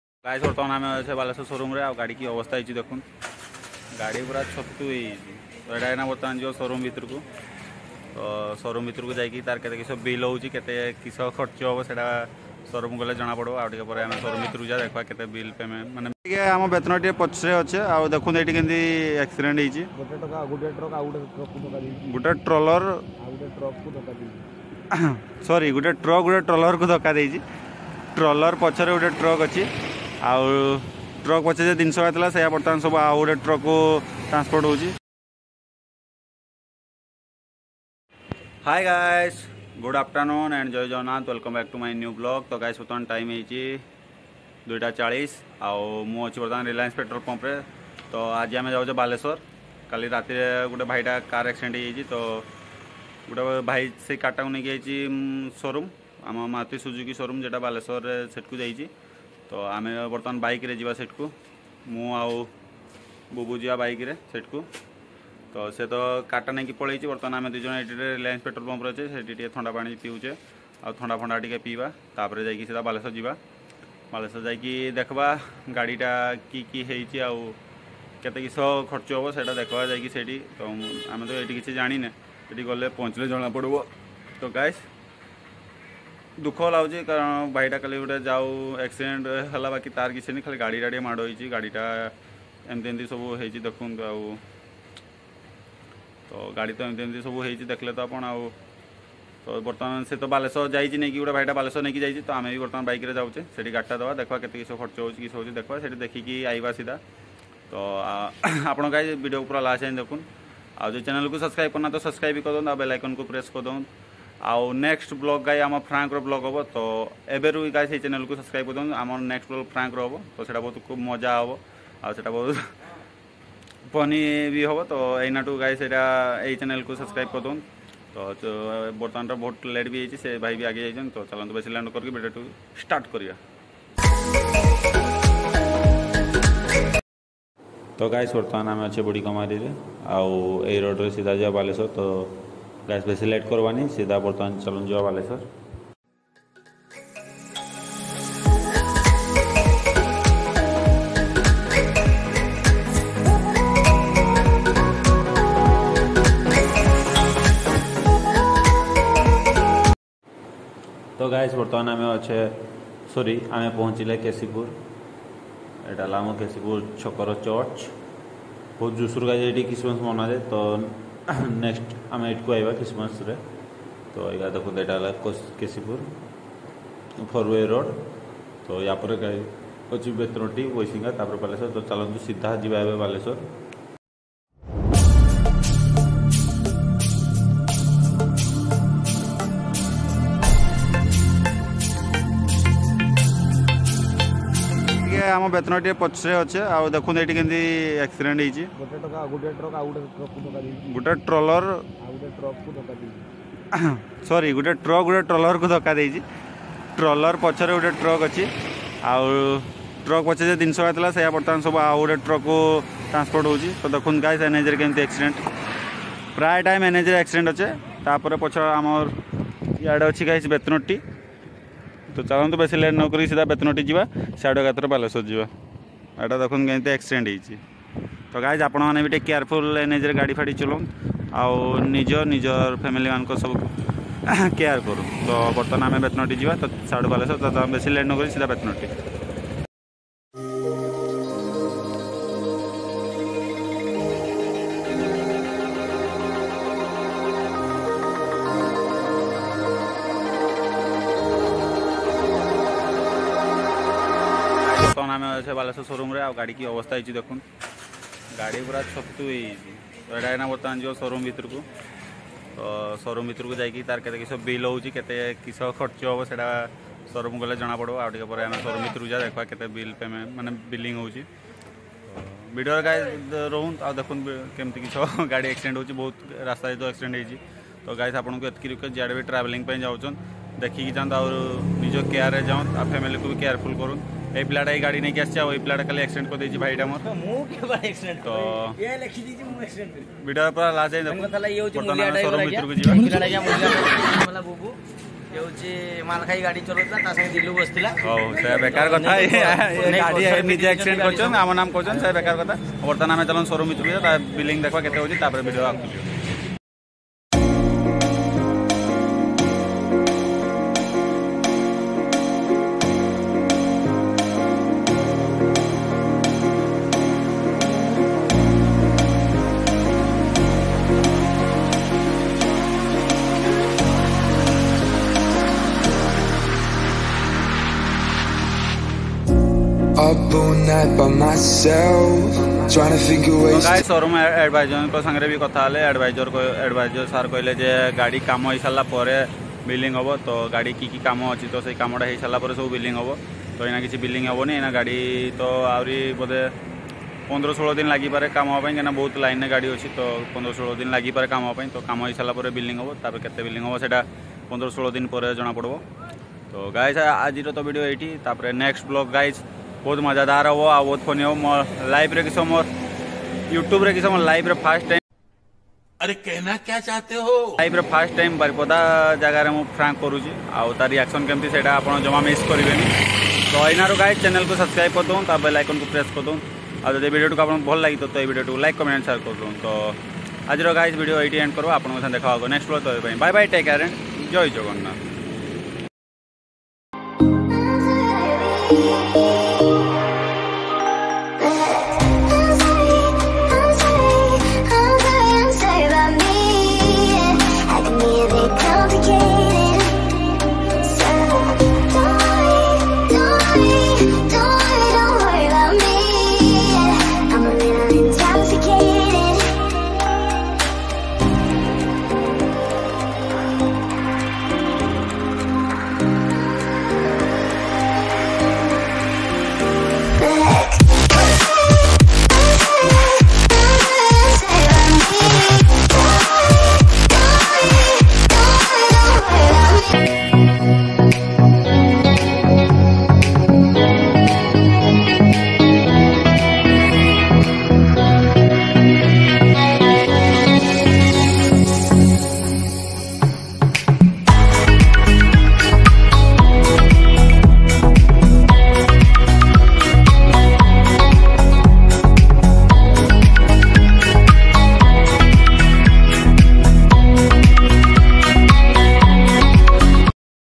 ROAD ACCIDENT/ମୋ ଭାଇ ର ACCIDENT ହୋଇଗଲା 😰🥺/odiavlogs
ROAD ACCIDENT/ ମୋ ଭାଇ ର ACCIDENT ହୋଇଗଲା 😰🥺/TO ସବୁ ଭାଇ ଆଉ ଭଉଣୀ ମାନଙ୍କୁ ଅନୁରଧ ଆପଣ ଯେବେ ଭି କୁଆଡେ ଲଙ୍ଗ ଯାଉଛନ୍ତି ଟିକେ ନିଜର ସେପିଟି ଦେଖୁତିବେ / ଧନ୍ୟବାଦ 🙏